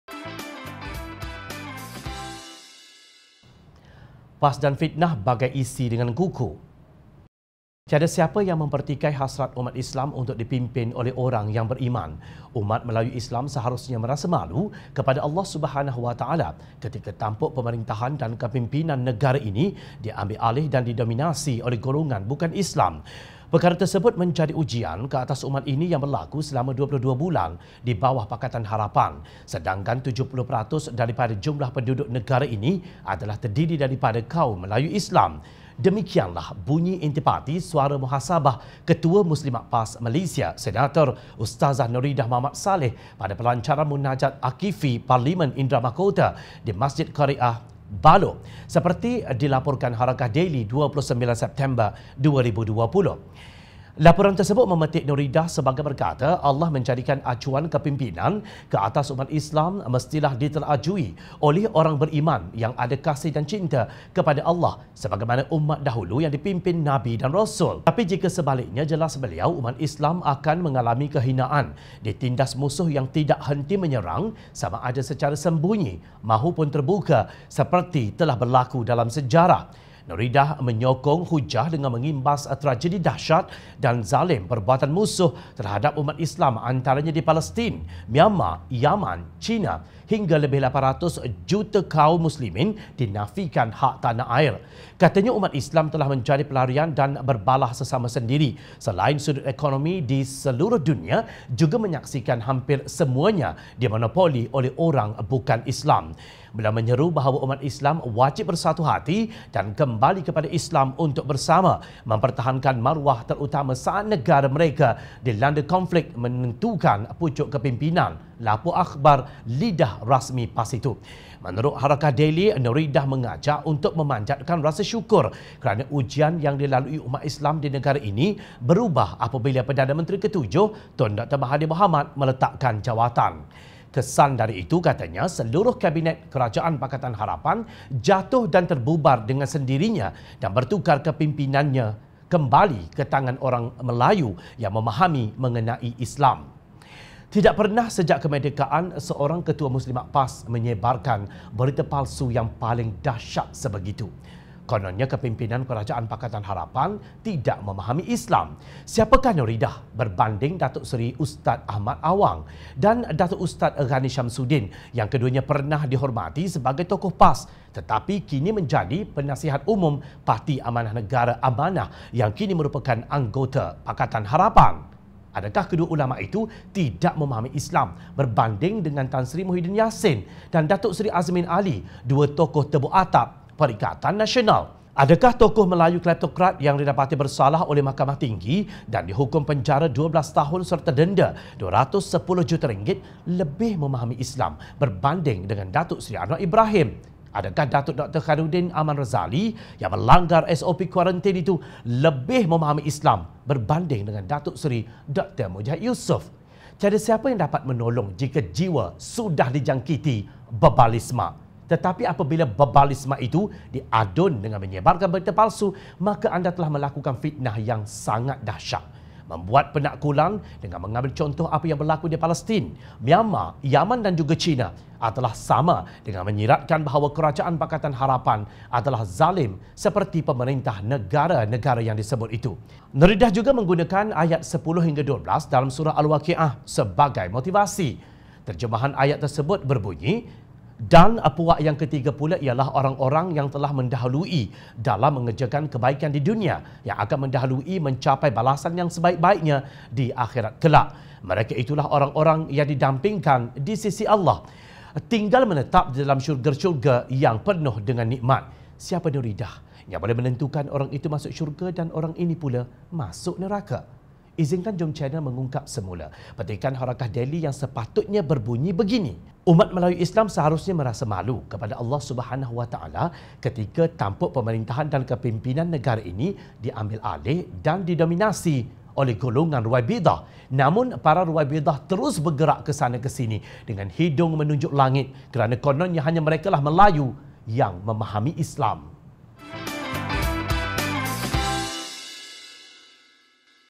PAS DAN FITNAH BAGAI ISI DENGAN KUKU
Nuridah menyokong hujahnya dengan mengimbas tragedi dahsyat dan zalim perbuatan musuh terhadap umat Islam antaranya di Palestin, Myanmar, Yaman, China hingga lebih 800 juta kaum Muslimin dinafikan hak tanah air.

Katanya umat Islam telah menjadi pelarian dan berbalah sesama sendiri, selain sudut ekonomi di seluruh dunia juga menyaksikan hampir semuanya dimonopoli oleh orang bukan-Islam.